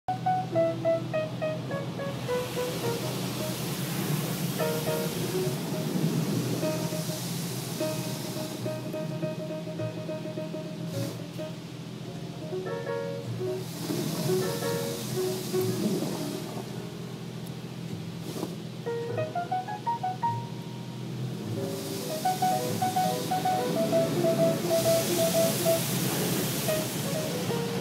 Come Clean by Cynthia Von Buhler
The first in a series of everyday events as art. Car Wash.